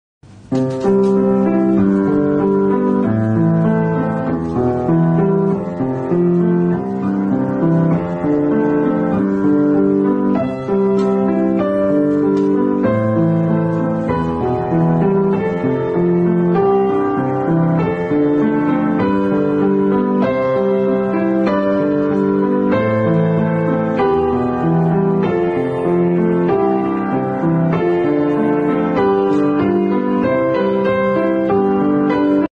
Let’s take a look at the unprocessed thermos cup:-)
Do you know what the unprocessed thermos cup looks like? Let's find out in this video